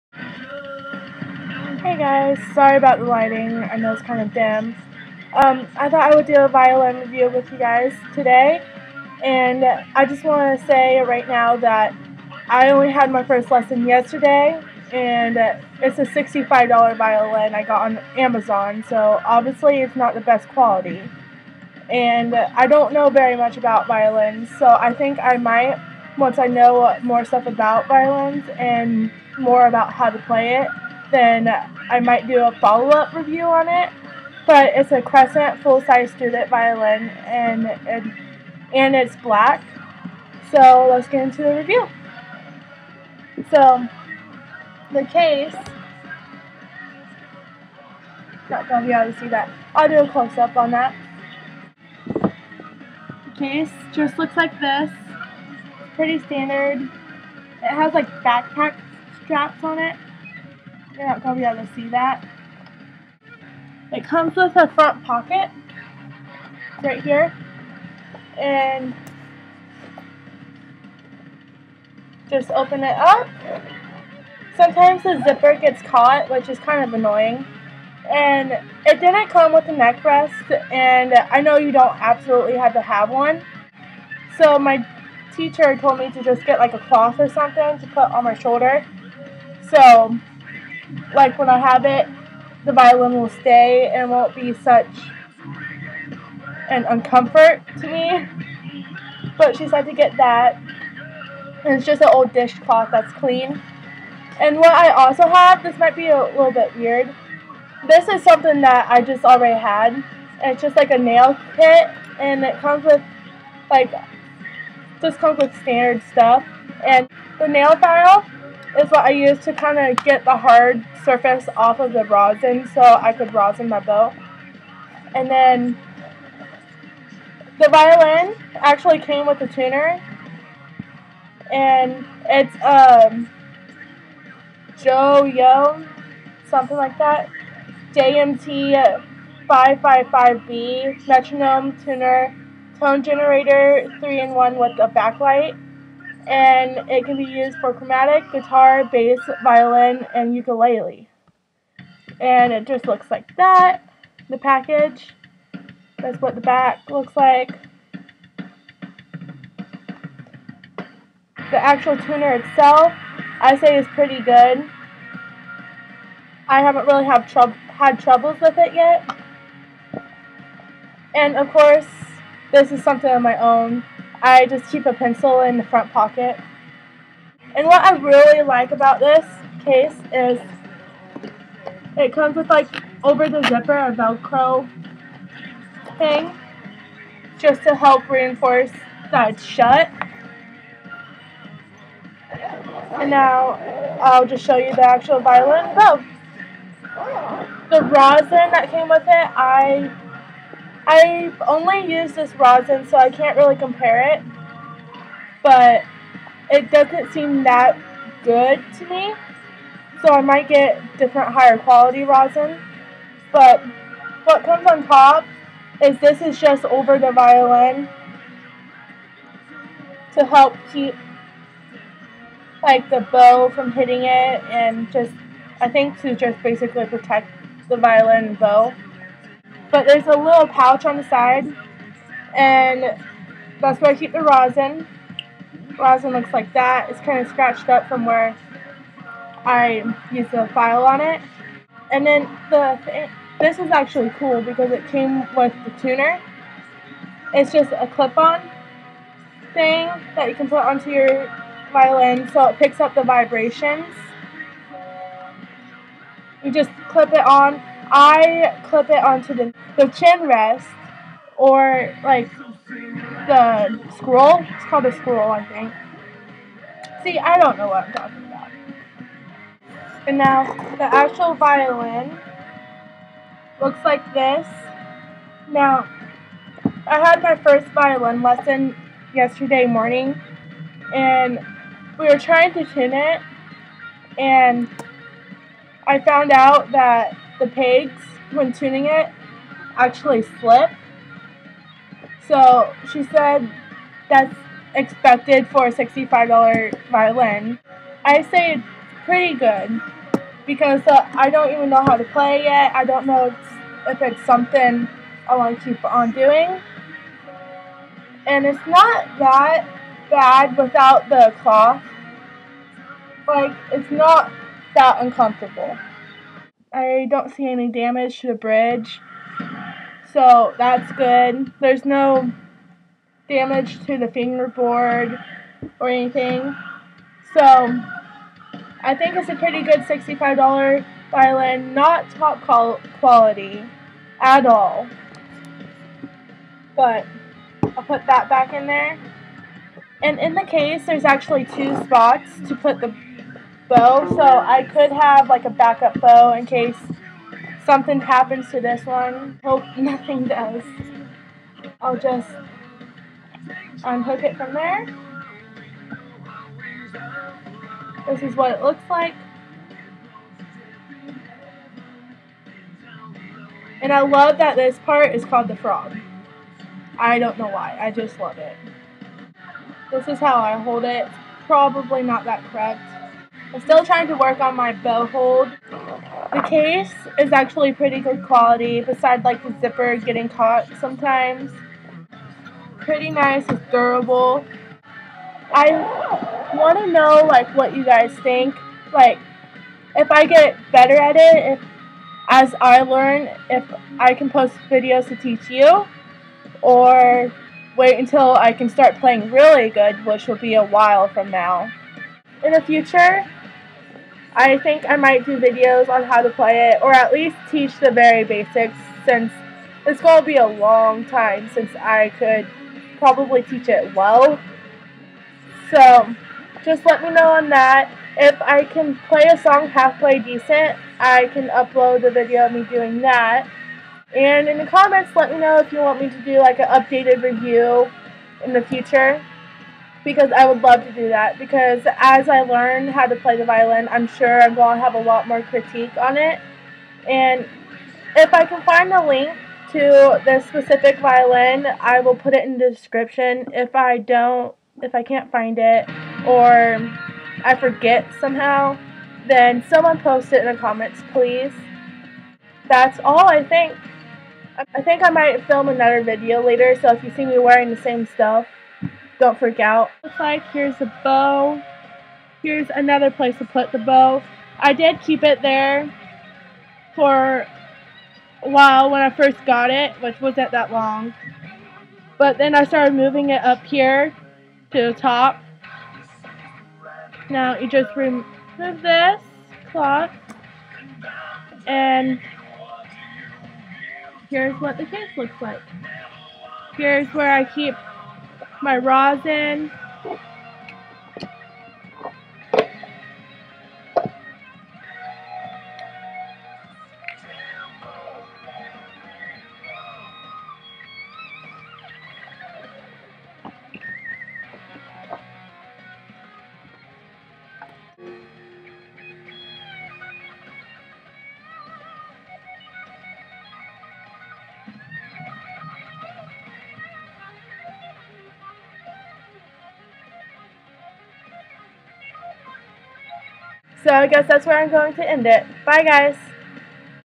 Crescent 4/4 Full Size Student Violin (Black) Review
Crescent Violin Links to purchase. I included a couple.

This is a review from a complete beginner. I've never even seen a violin before (except online) I got this one.